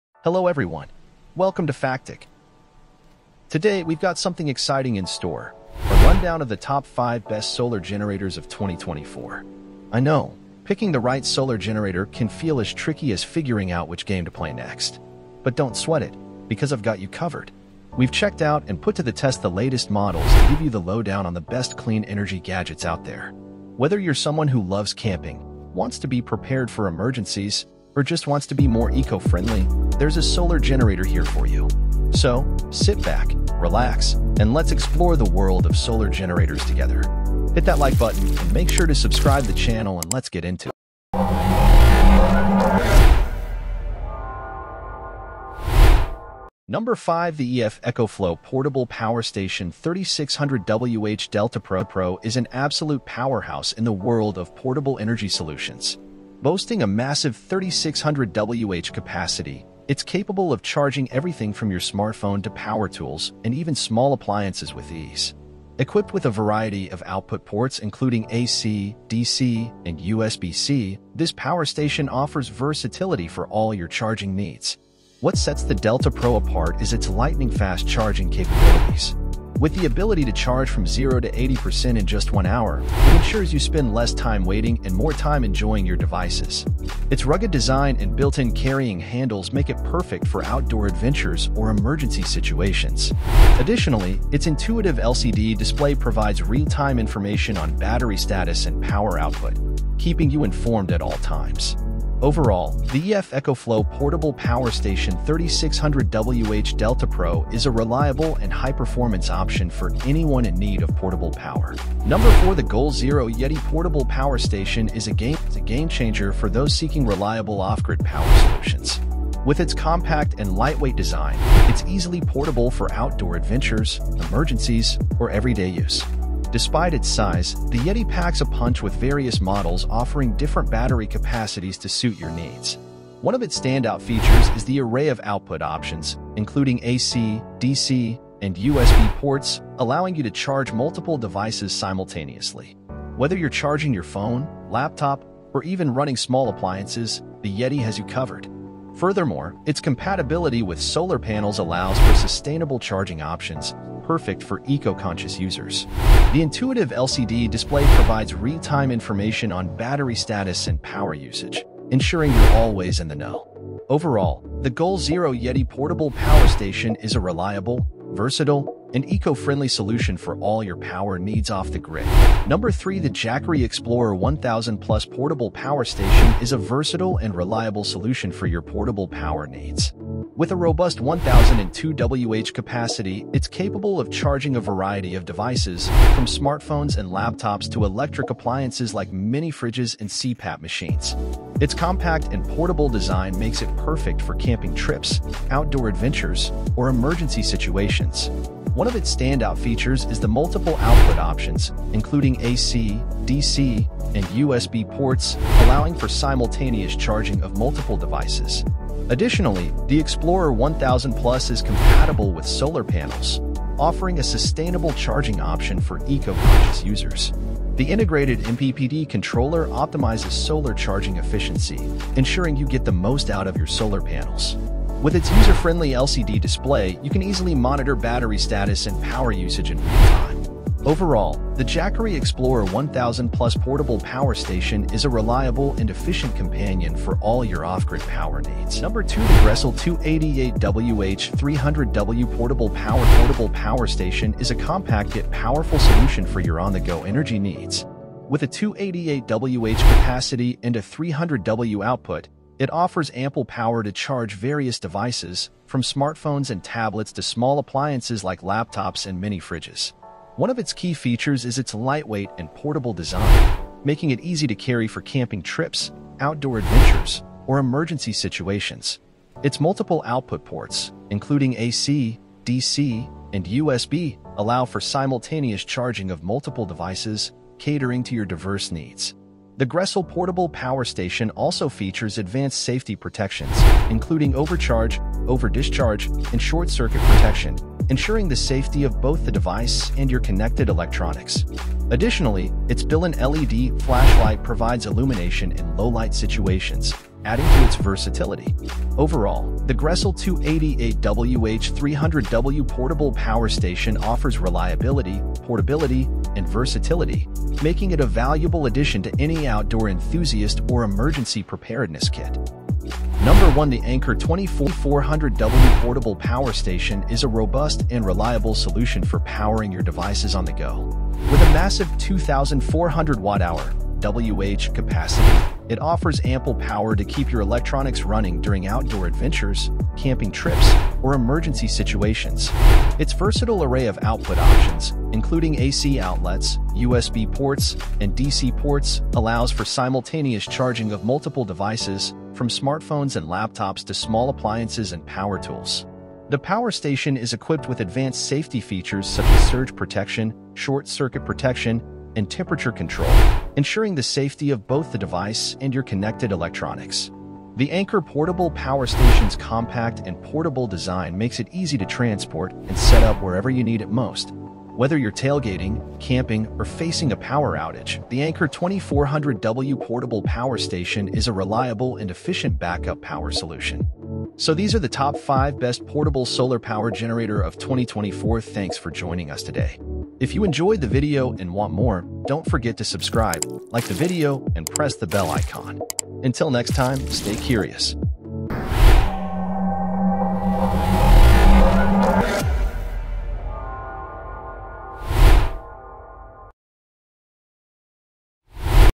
Top 5 Best Portable Power Stations 2024 - Ultimate Guide
Top 5 Best Solar Generator of [2024]

Looking for the best solar generator for your next outdoor adventure or emergency situation? Check out our ultimate guide to the Top 5 Best Power Stations of 2024. From portable options to heavy-duty power sources, we've got you covered! Don't wait until it's too late - be prepared with one of the best solar generators on the market.

➜ Here are the Top Best Solar Generator featured in this video:

✅4. Goal Zero Yeti 1500X
✅3. Jackery Explorer 1000 Plus

Remember the hassle of dealing with noisy, fuel-guzzling backup generators during power outages? Those bulky machines, requiring setup outside to avoid filling your home with toxic fumes, were far from convenient. Now, envision a world powered by solar energy, where reliance on a solar generator reigns supreme.
This innovative device merges the capabilities of a backup battery with the efficiency of solar panels. It's akin to having a silent, eco-friendly guardian ensuring your home stays lit and your essentials remain cool, uninterrupted by power failures. Welcome to the future of backup energy.

Table of Contents:

0:00​​ - Introduction
0:47 - EcoFlow DELTA Pro
1:52 - Goal Zero Yeti 1500X
2:53 - Jackery Explorer 1000 Plus
3:59 - GRECELL T-300
5:13 - Anker SOLIX F2000
6:14 - Conclusion

Hope you enjoyed my Top 5 Best Solar Generator of 2024 video.